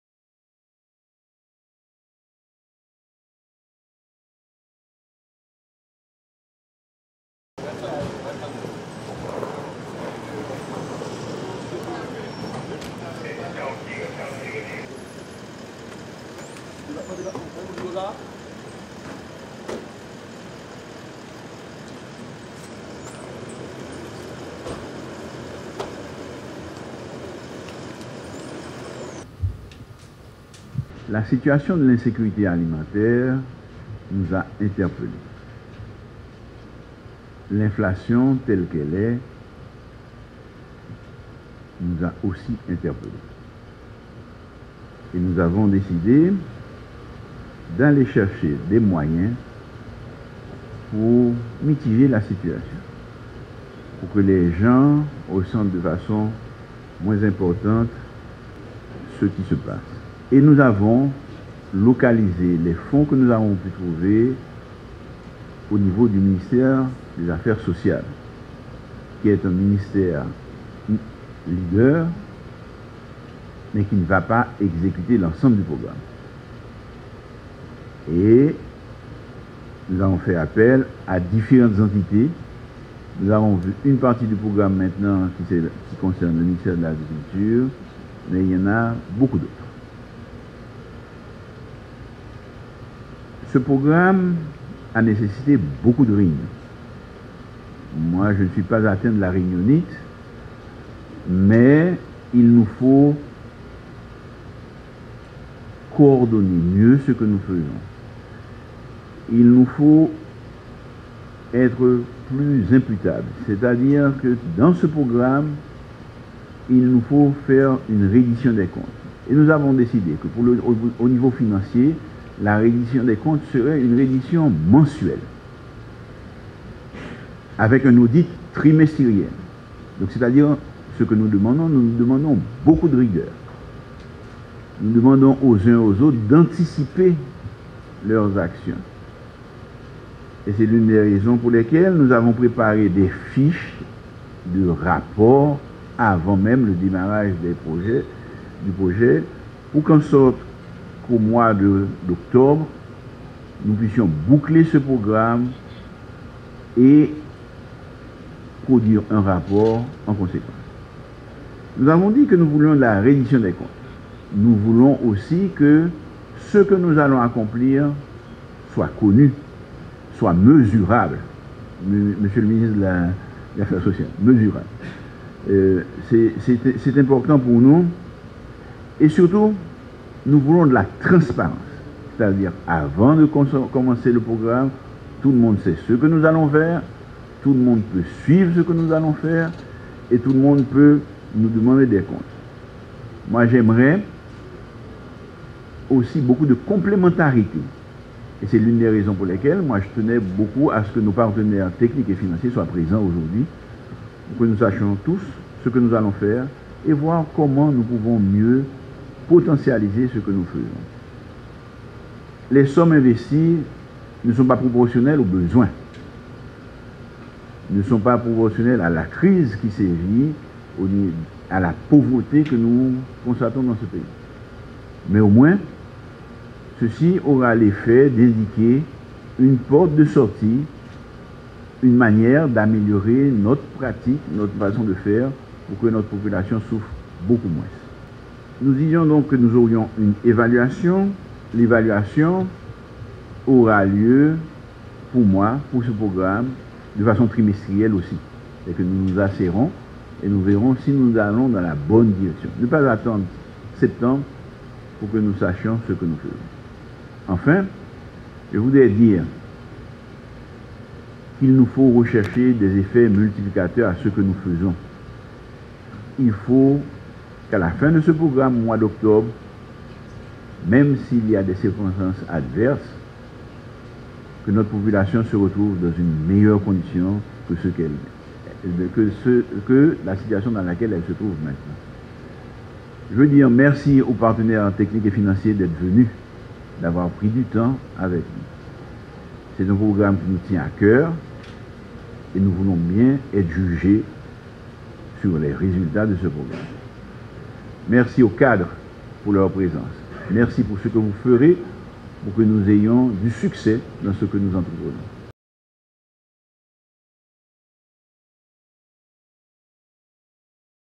Réunion de travail autour du sous programme sectoriel d'appui à la sécurité alimentaire
Le Premier ministre, Dr Ariel Henry, a présidé, ce mercredi au ministère de l'Agriculture, une importante réunion de travail autour de l'approfondissement du document du sous programme sectoriel d'appui à la sécurité alimentaire et nutritionnelle